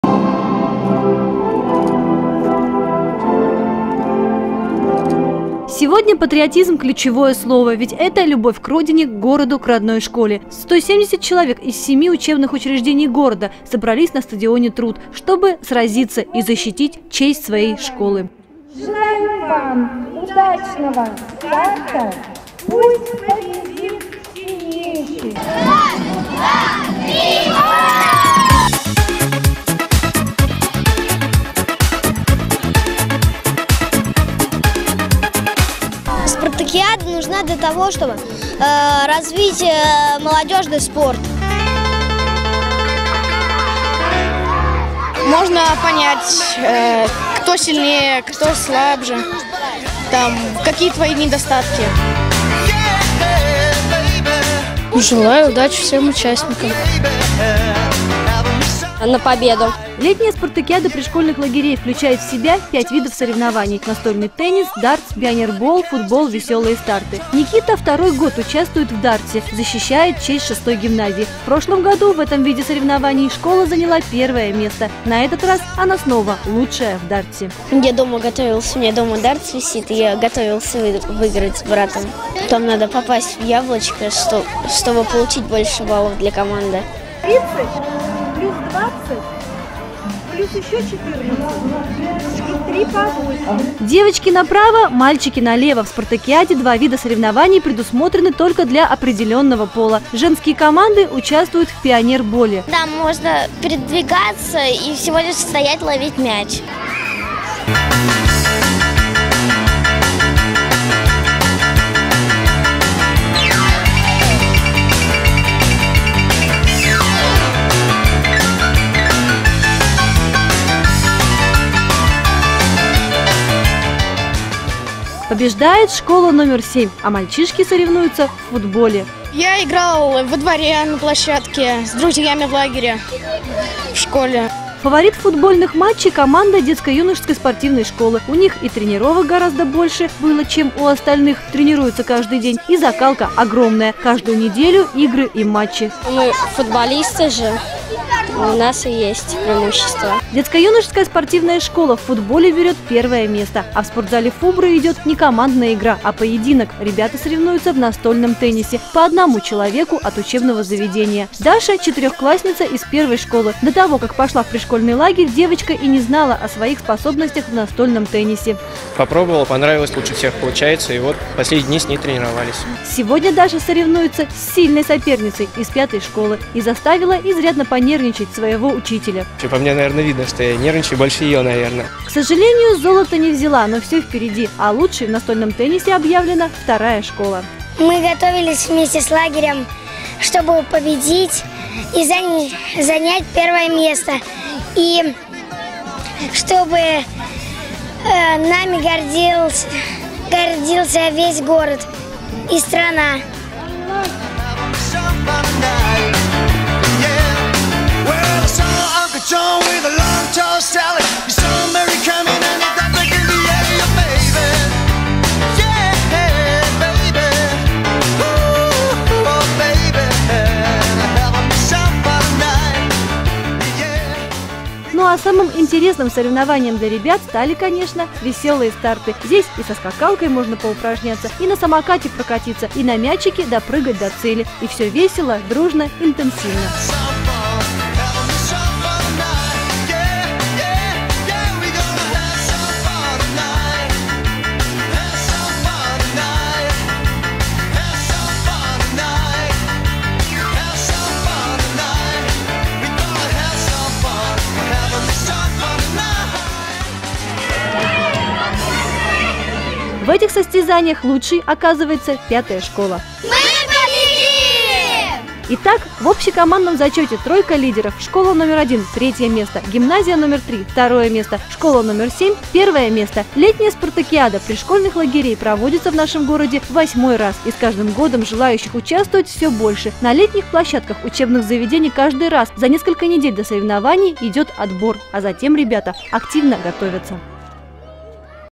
Летняя спартакиада пришкольных лагерей Ивантеевки на призы Совета депутатов
В восьмой раз в Ивантеевке проводится летняя спартакиада пришкольных лагерей на призы Совета депутатов. Наши школьники активно к ней готовятся, выбирают вид соревнований, тренируются, чтобы затем доказать, их образовательное учреждение — самое спортивное.